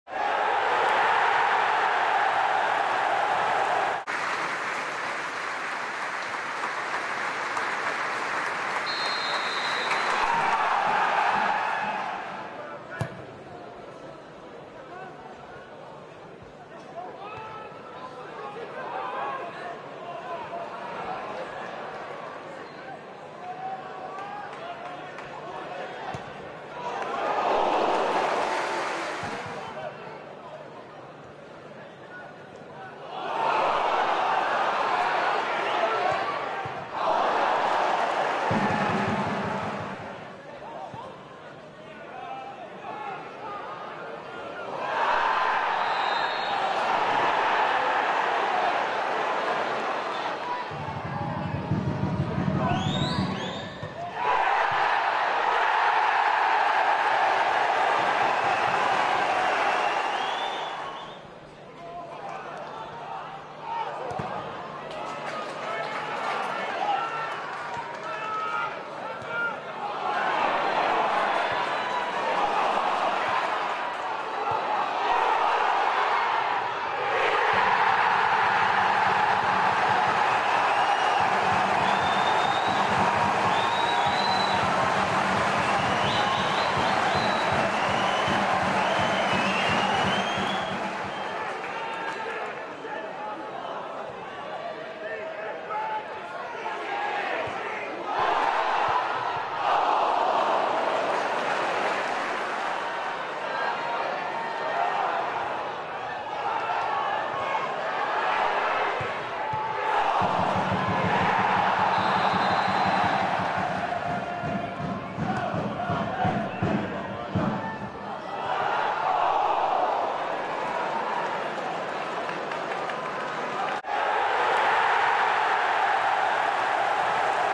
HIGHLIGHTS: Bury 1 Town 0
Short highlights of the 1-0 defeat away to Bury